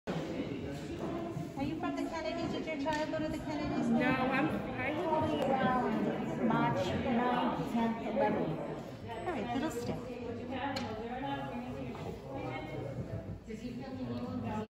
100 Billerica residents vaccinated against COVID-19
The town of Billerica hosted their second COVID-19 vaccine clinic on Thursday, Feb. 11, at the Town Hall.